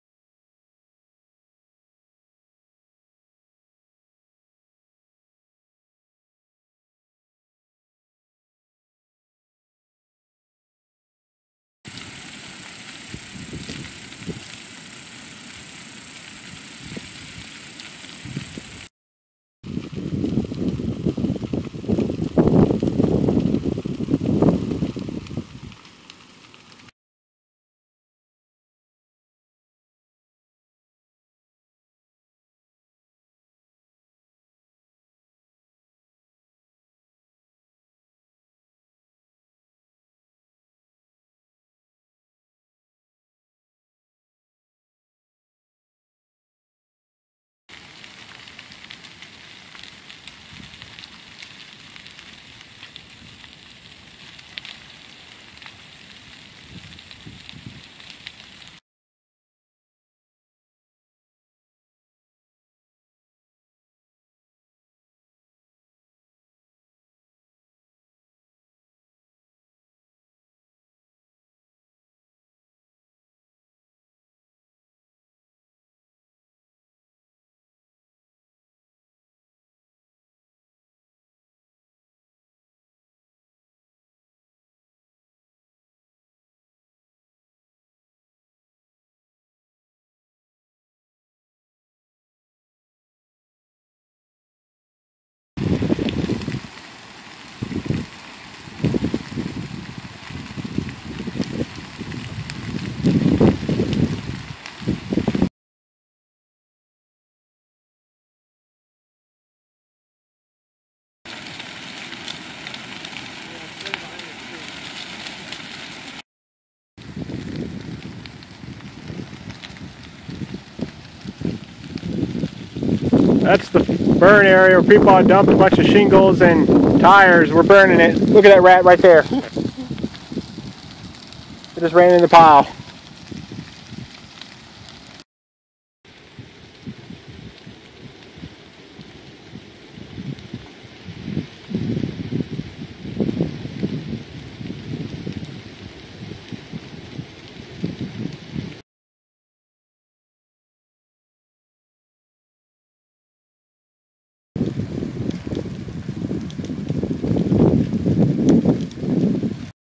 Farm center section burns
My Dad and I burning the center section of the farm between the east and west fields.